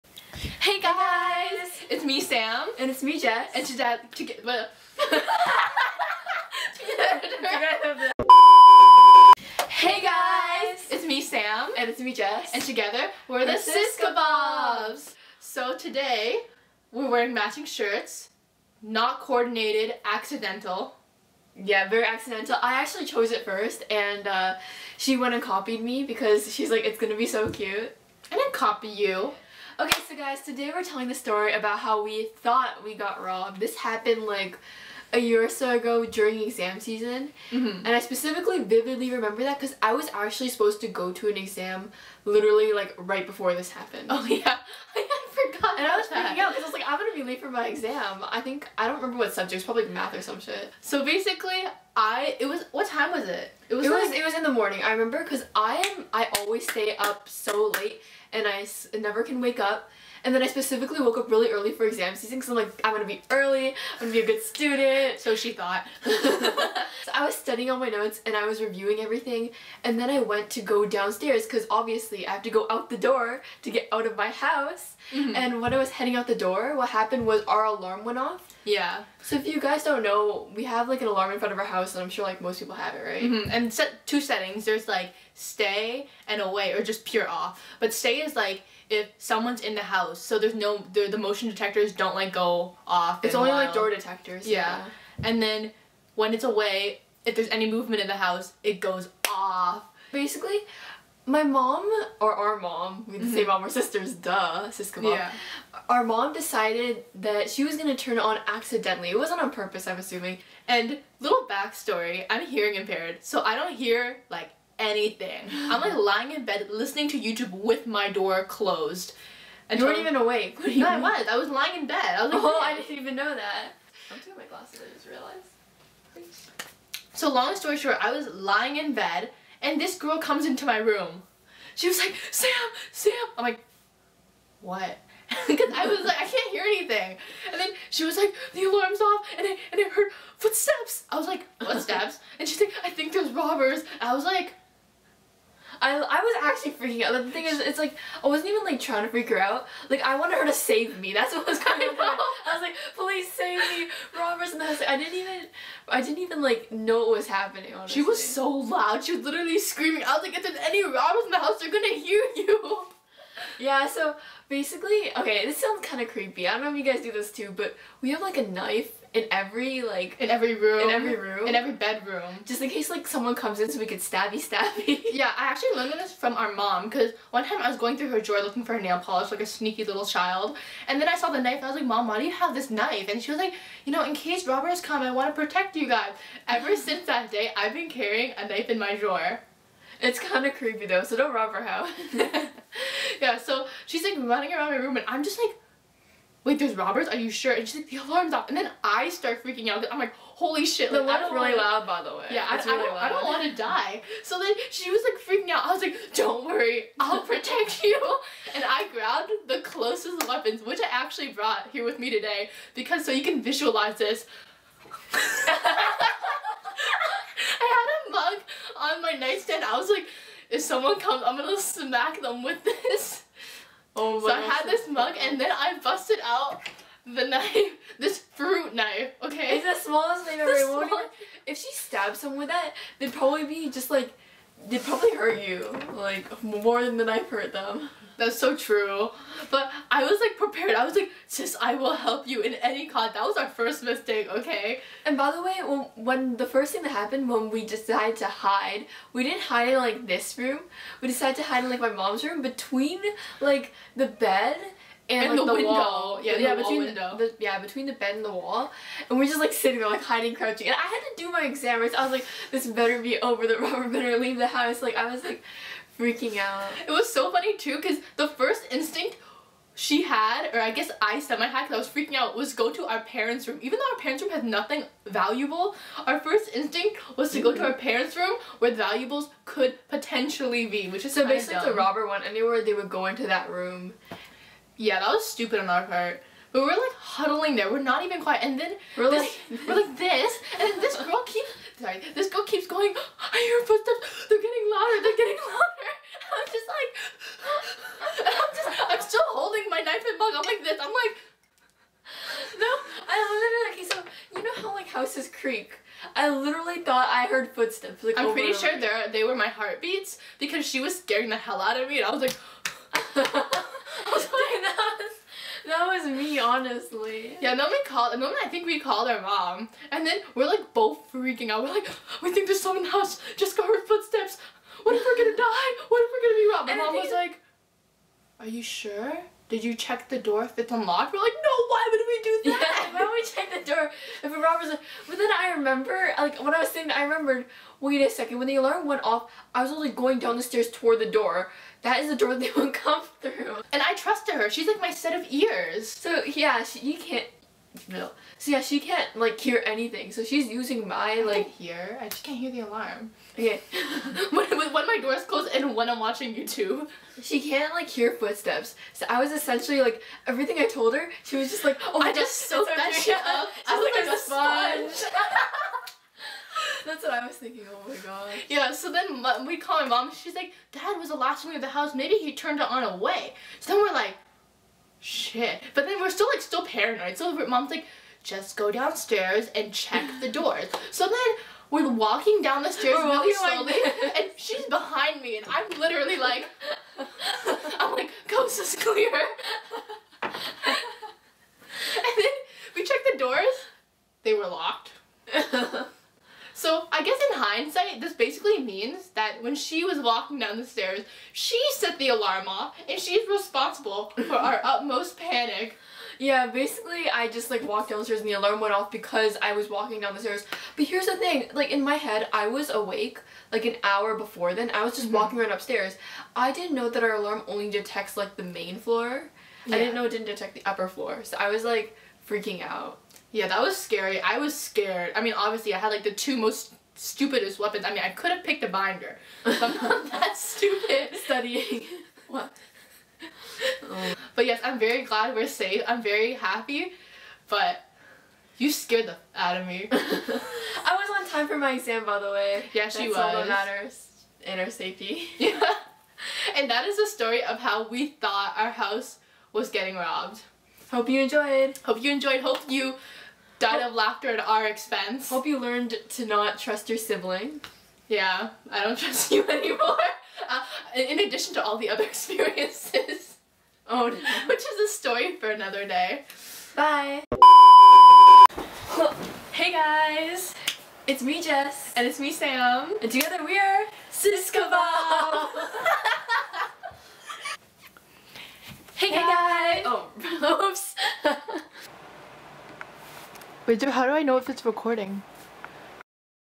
The time we thought we were getting robbed | Storytime
Today we decided to tell the story of the time where we thought our house was being broken into/getting robbed.  Although we were really scared and probably close to tears in the moment, we laugh about it now because there were never any robbers in the house and our logic during the whole story was kind of flawed.  Anyways we are extremely grateful it was a false alarm, so hope you enjoy!